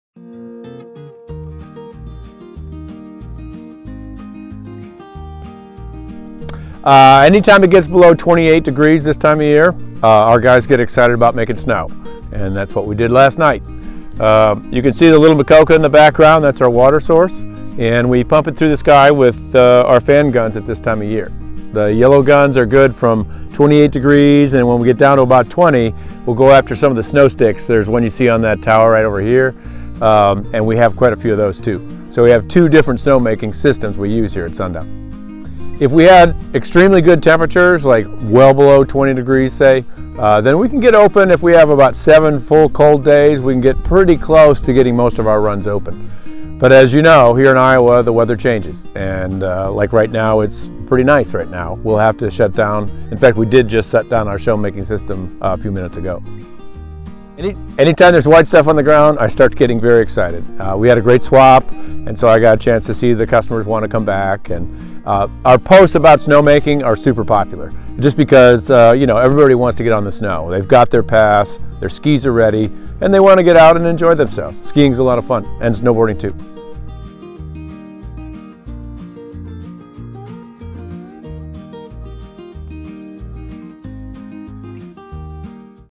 SNOWMAKING: How It Works at Sundown Mountain Resort in Dubuque, IA
❄️SNOWMAKING: How It Works at Sundown Mountain Resort❄️

It's that time of year when you see the snow falling on the hillside! Our GM, Mark Gordon, gives us a glimpse of what goes into snowmaking here at Sundown Mountain Resort in Dubuque, IA.

And special thanks to our Snowmaking Crew for making this all happen. They work long hours to bring you outdoor fun on our slopes.